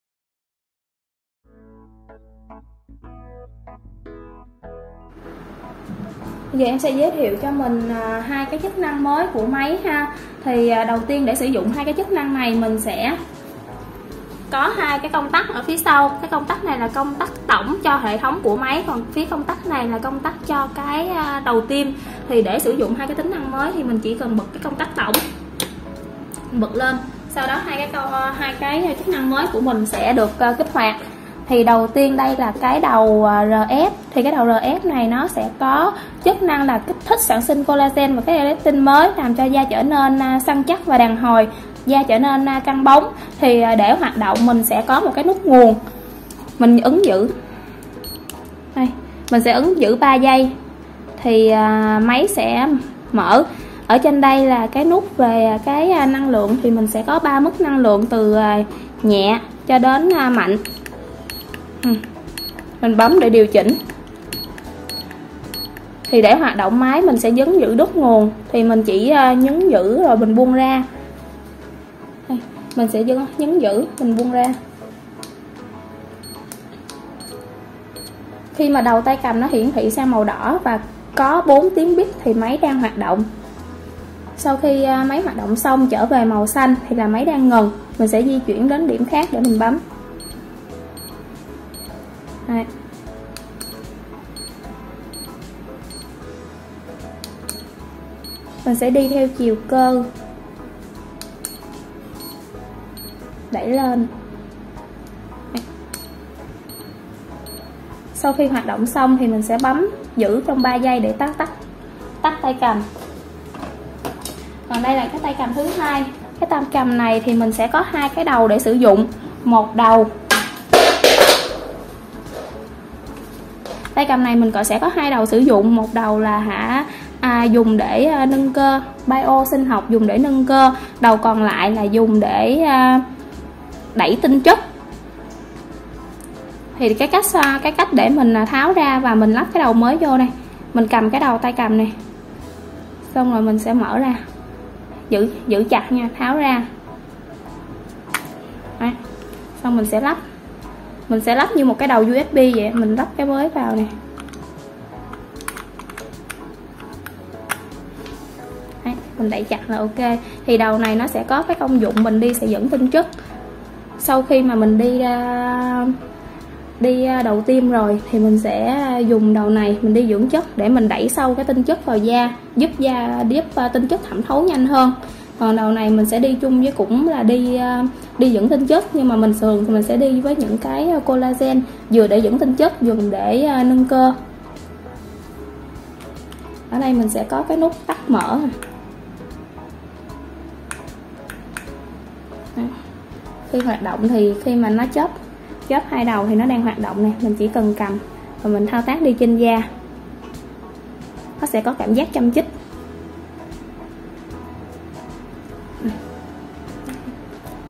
2 tính năng máy Tiêm dưỡng chất Hycoox Injector
Hycoox Injector là thiết bị tiêm tiên tiến, với nhiều ưu điểm vượt trội
😘😘Ưu điểm vượt trội của HYCOOX :
☝️Đầu tiêm với 9 mũi kim tự động có thể điều chỉnh mức độ tiêm mạnh nhẹ tùy theo từng loại da.
✌️Thao tác sử dụng đơn giản vì có chức năng cảm biến
🤟Không gây tác dụng phụ hay phản ứng phụ
Máy được tính hợp 2 tính năng mới:
- Đầu kích thích sản sinh collagen, tăng đàn hồi cho da
- Đầu nâng cơ, đẩy tinh chất sâu vào da, nuôi dưỡng làn da tươi trẻ
Cùng khám phá 2 tính năng mới của Máy tiêm dưỡng chất Hycoox Injector trong video này của THANH THÚC nhé!
THANH THÚC GROUP - Chuyên Cung Cấp Thiết Bị Spa Uy Tín, Chất Lượng
🏢 Địa chỉ: 182 Thành Thái, Phường 12, Quận 10, Thành phố Hồ Chí Minh